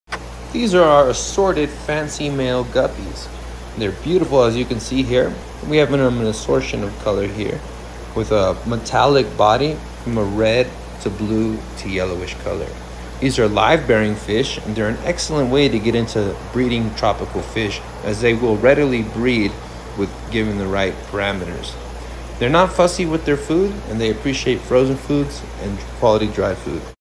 Guppy: Assorted Males
assorted Guppy: we have many high quality guppies in stock! These assorted Guppy are fancy guppies with great colors and are ready for a planted tank or breeding aquarium. As livebearers Guppies give live birth to fully swimming fry, a great way to get introduced into how to care for fry and understand Guppies. Great price and beautiful colors the assorted options on our site are unmatched.